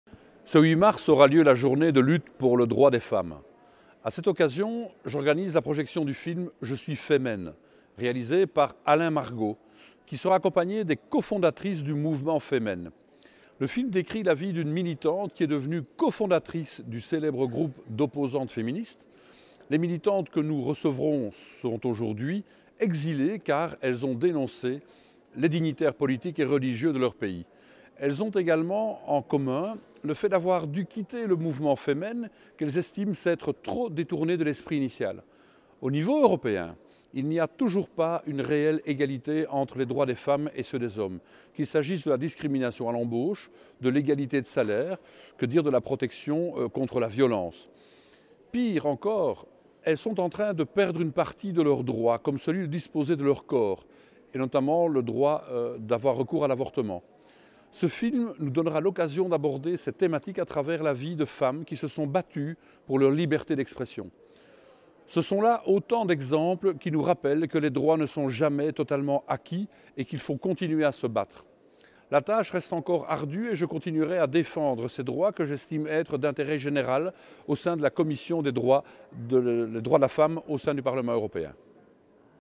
La co-fondatrice des FEMEN témoignera au Parlement européen
Ce 7 mars, en tant que membre de la commission droit des femmes et égalité des genres, je propose la projection du film « Je suis Femen » en présence du réalisateur et de l'actrice principale. La diffusion de ce film est une première au sein d'une institution politique internationale. Elle sera l'occasion d'aborder le recul des droits des femmes et de la liberté d’expression. Nous entendrons aussi le récit de cette femme, Oksana Shachko, cofondatrice d'un mouvement FEMEN qu'elle a préféré quitter et exilée du pays où elle est née.